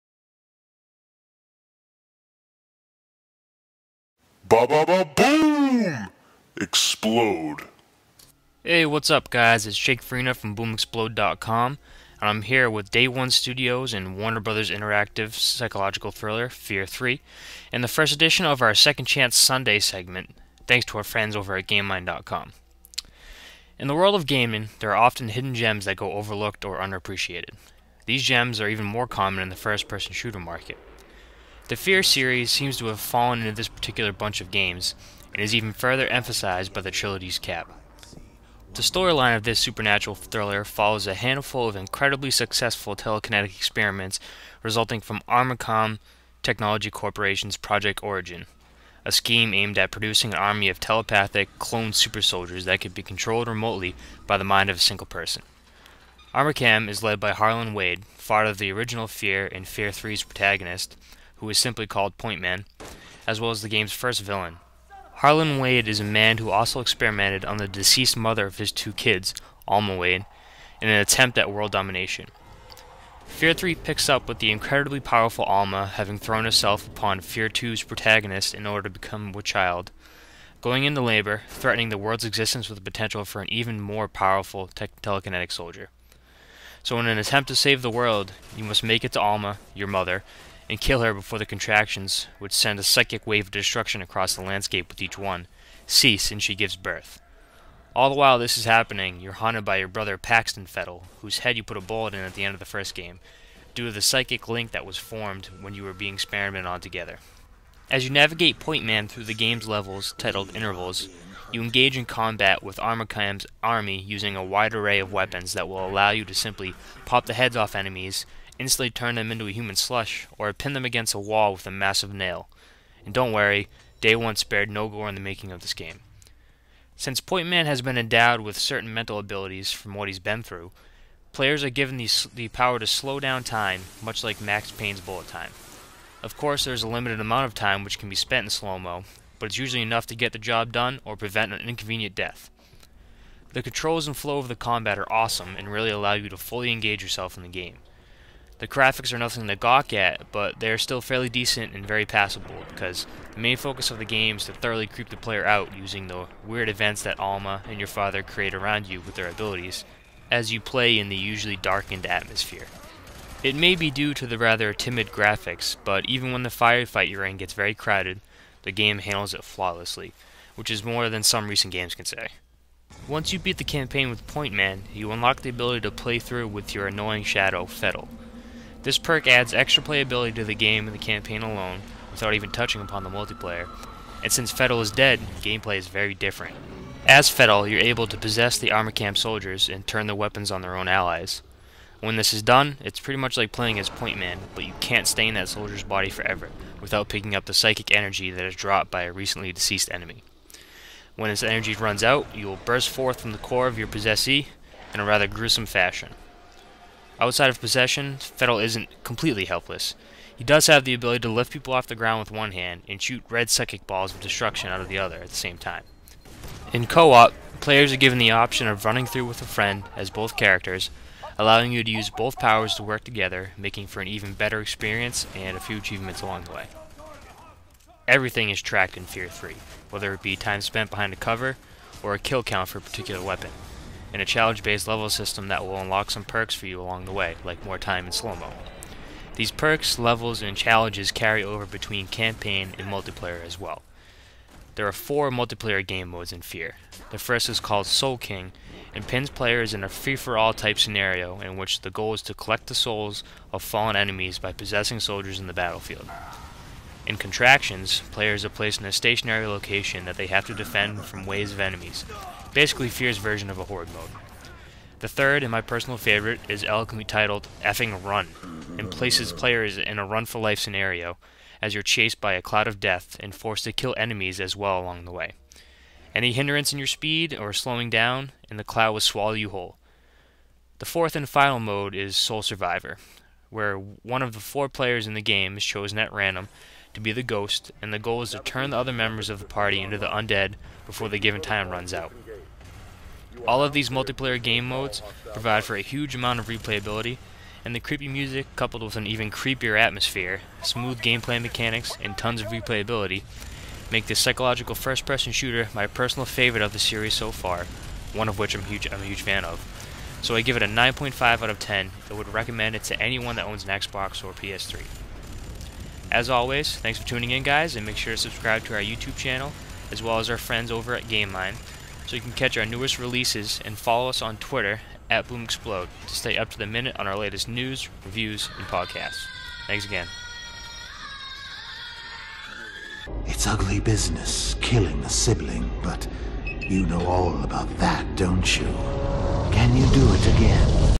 Second Chance Sunday Reviews: F.3.A.R.
This is the first edition of our Second Chance Sunday segment and video review of F.3.A.R., released on June 21, 2011.